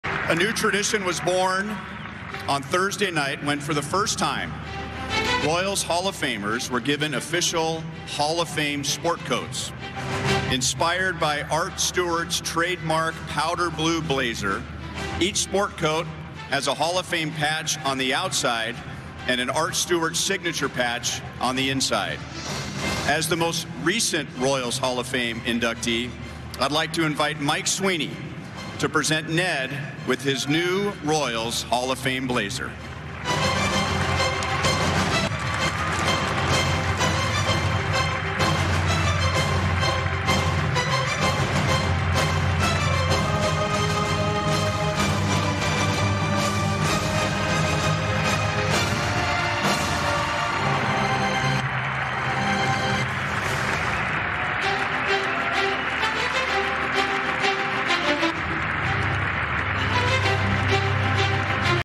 Ned Yost recieves his Royals Hall of Fame blazer
The newest member of the Royals Hall of Fame: Ned Yost.